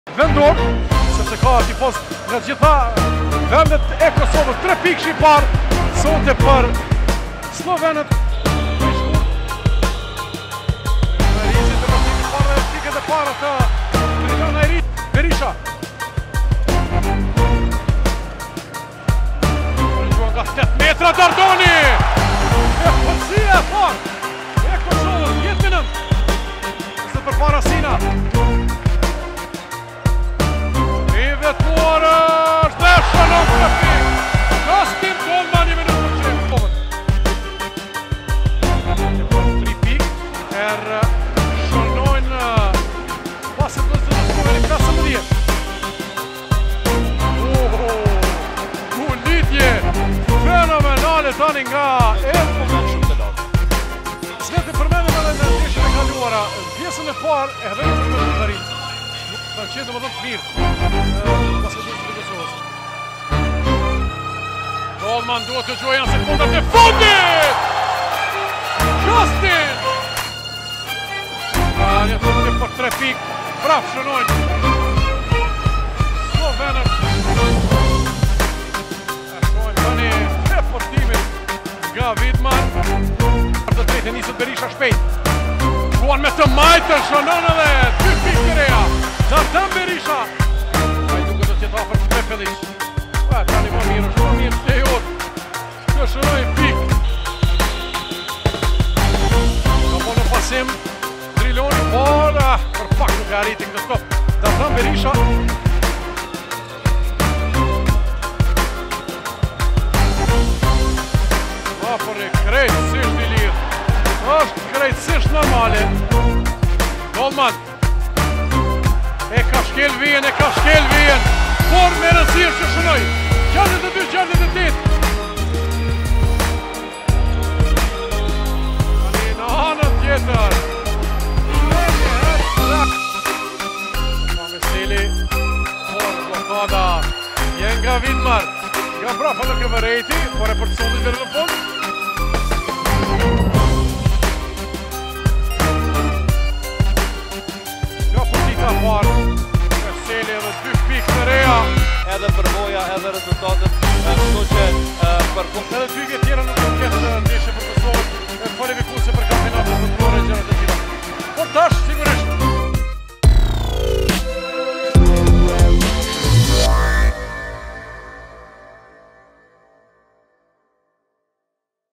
Kosovo v Slovenia - Highlights - FIBA EuroBasket 2017 Qualifiers
Check out the highlights of Kosovo v Slovenia  from the FIBA EuroBasket 2017 Qualifiers.

FIBA EuroBasket 2017 will take place in Finland, Israel, Romania and Turkey. Follow the qualifiers on our YouTube channel!